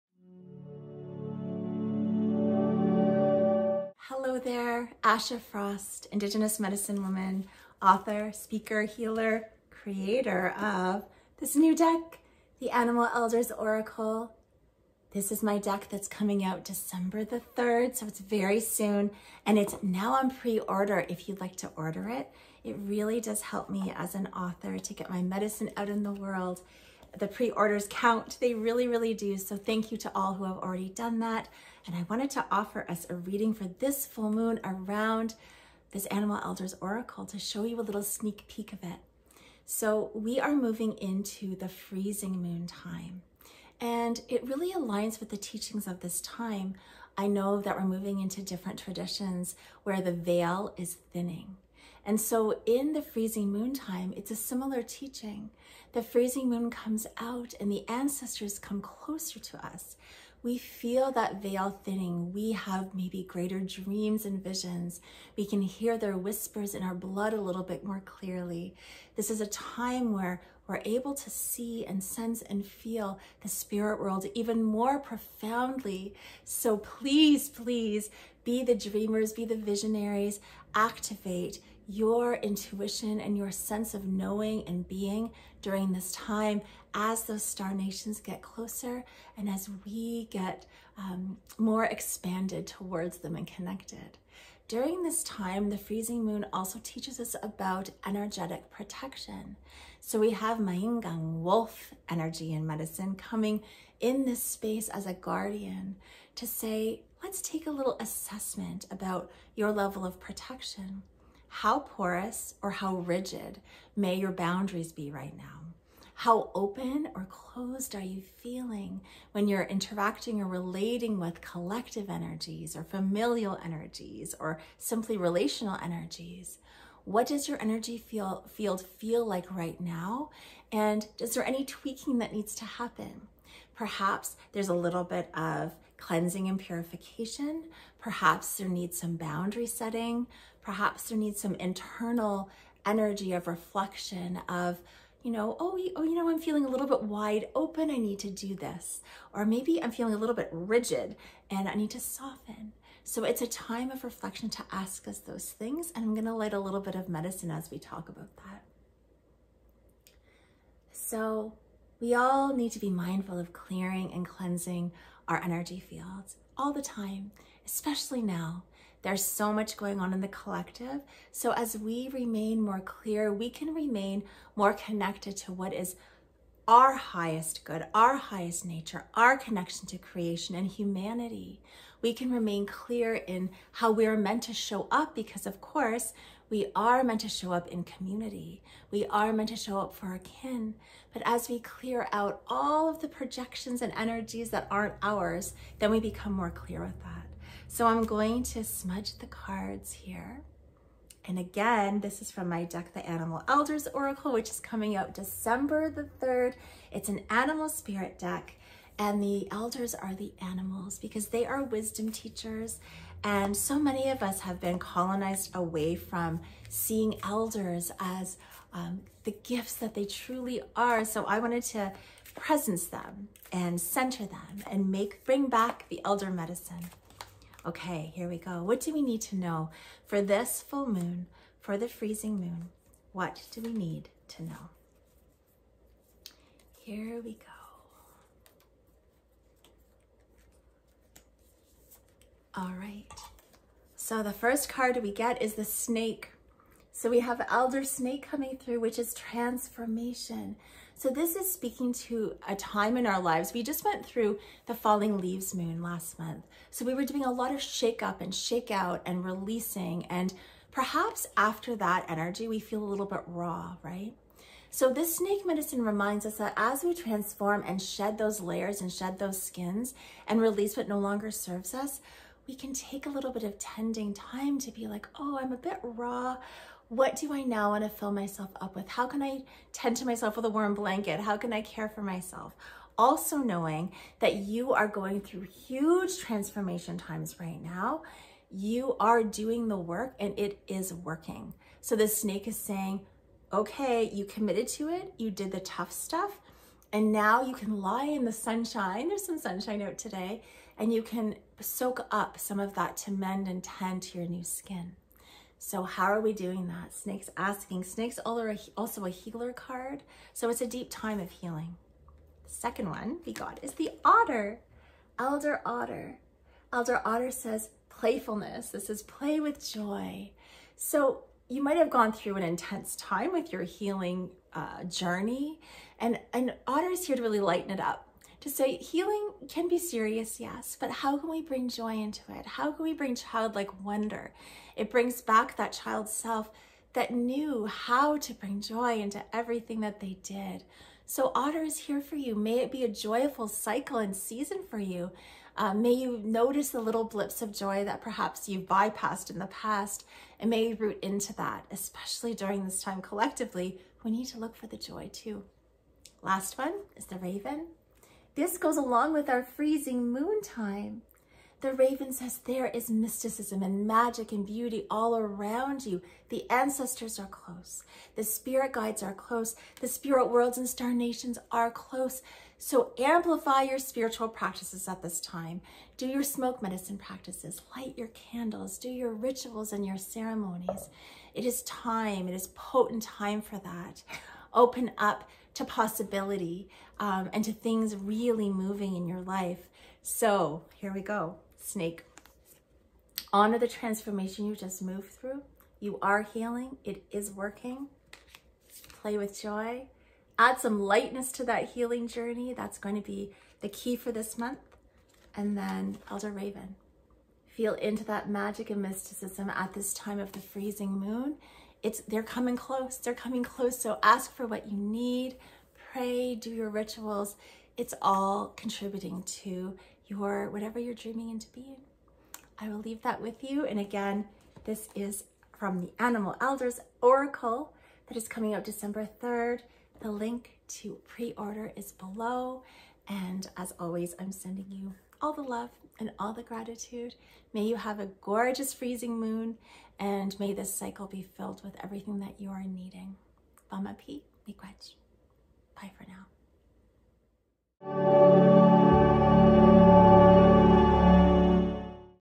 October FULL MOON 3-Card Oracle Reading
“Our eleventh Moon of Creation occurs  when the Star Nations, the Cosmos, and the support from the Upper Worlds are near. With the Star Nations close, sensitivity and intuition are heightened, and all our senses are on alert.

Our energy fields are highly attuned during this Moon cycle, so we learn how to protect our dreams, energy, and hearts. Energetic boundaries are an essential part of Creation, and this Moon reminds us that the more we are aware of the impact of our thoughts, actions, and intentions, the more we can walk in alignment with our great vision.

The Stars are here to support our well-being, and they wrap us in protection and love.”
- An excerpt from You are the Medicine

Books + Oracle Decks
Amazon USA
Amazon Canada
***The Sacred Medicine Oracle

🦬🦅What is your Animal Spirit? 🦌🐻